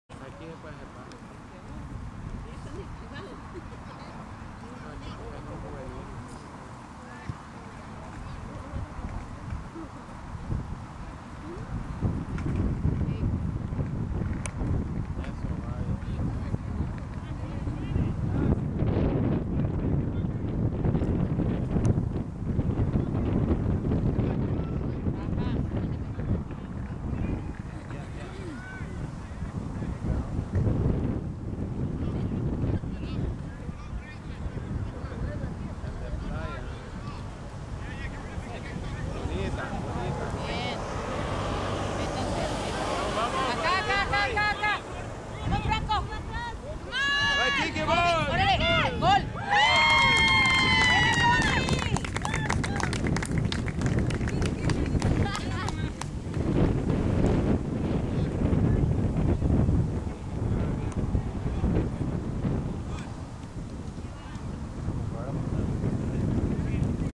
AHFC96B vs Texans North goal 1 by Cody Fielden Texas Warm Cup 2013
Albion Hurricanes F.C. - AHFC96B (U18) vs Texans North Championship game won 2:0 goal by Cody Fielden 8/25/13 M2U01898